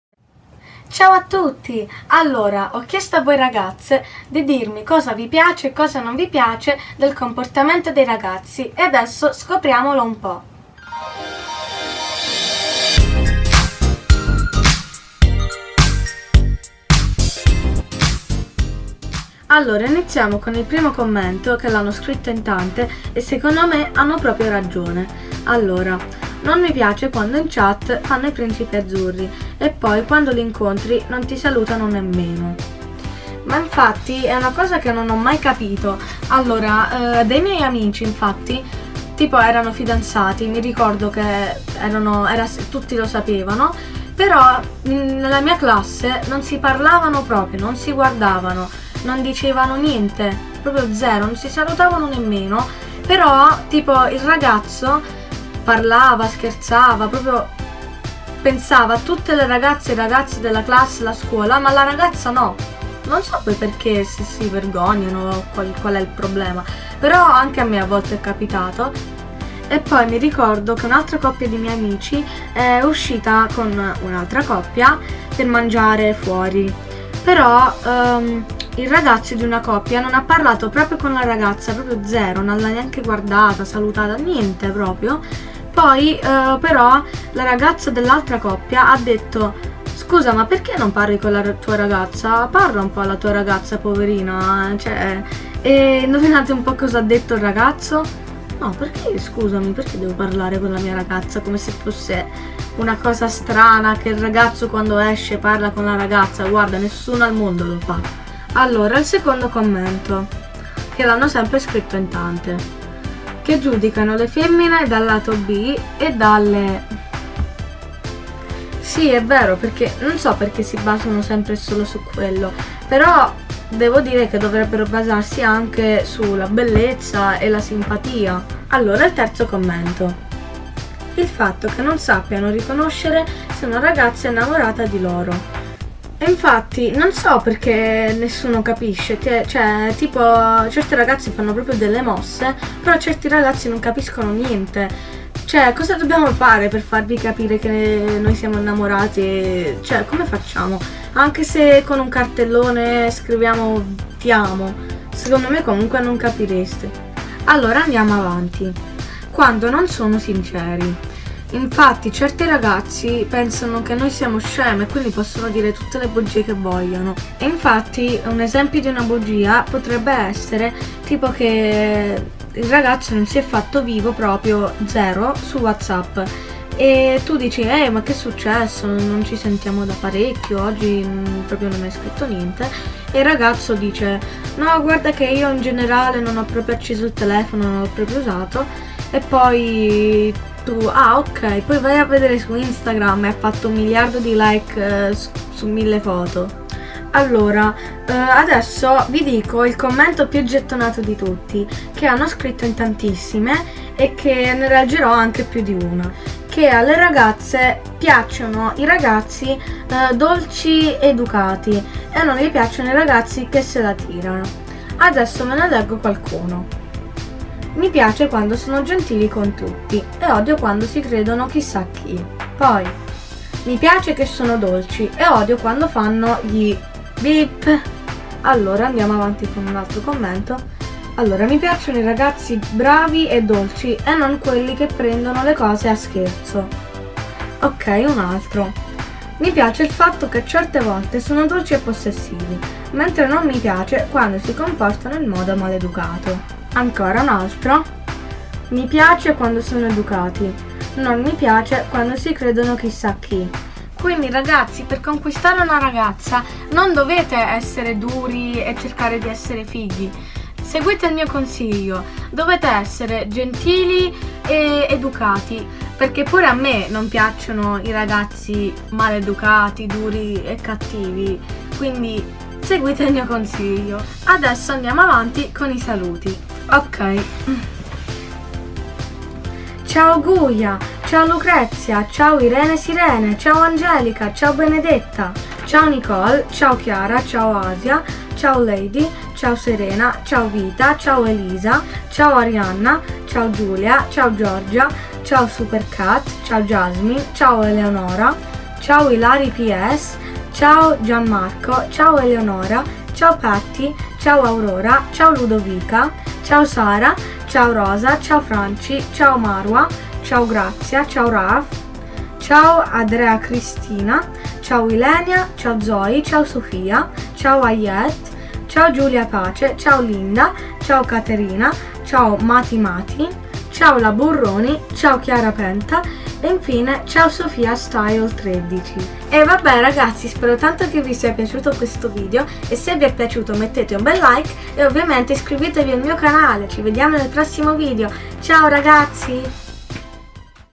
COSA PIACE ALLE RAGAZZE DEI RAGAZZI? L'ho chiesto a tante ragazze su Instagram! || SSnS
Alle ragazze piace…
Guardate il video per scoprirlo! ;)